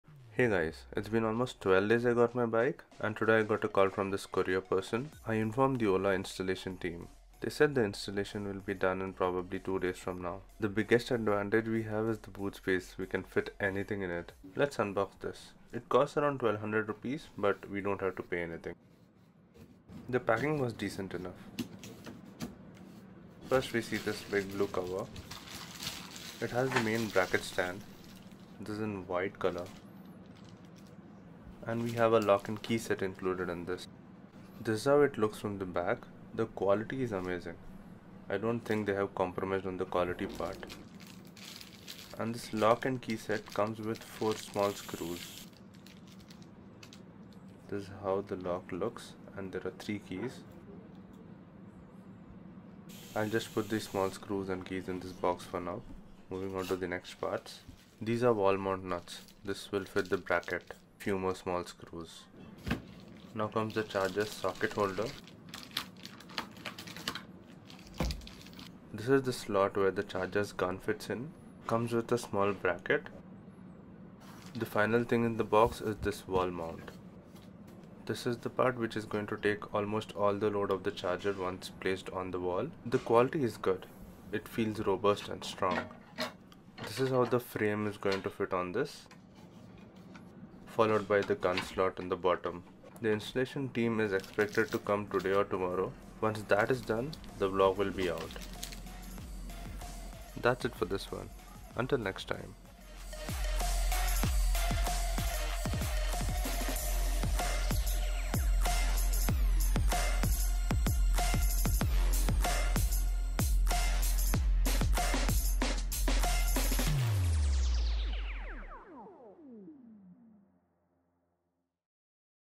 Ola Wall Mount Charger Bracket | Unboxing | OlaS1Pro
This video is the unboxing of ola electrics wall mount charger, Each and every part from the box has been clearly described. This will probably be helpful for anyone who has orders or planning to buy an ola electric scooter.

My Ola S1 Pro scooter was delivered yesterday (6th March, 2022) at around 10:05 AM in Hyderabad.

25th Aug 2021 - Reservation 500
15th Sep 2021 - Advance Payment 20,000
30th Jan 2022 - Full Payment 1,20,578
24th Feb 2022 - Charger Installation Payment 2,359

Below is my timeline update since dispatch:

28th Feb, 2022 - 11:17 PM
Dispatch Email

4th March, 2022 - 8:51AM
Text message from ICICI INSURANCE purchase update

4th March, 2022 - 3:00 PM
Got a call from temporary registration team, just to confirm address and RTO.

5th March, 2022 - 1:45 PM (Today)
Email from ICICI, with insurance document.

5th March, 2022 - 8:05 PM (Today)
Ola Registration, VAHAN OTP pending email.

6th March, 2022 - 9:22 AM
Got a call from Ola, My scooter delivery is scheduled today in next 2hrs.

6th March, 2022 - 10:05 AM
Delivery vehicle arrived at my location.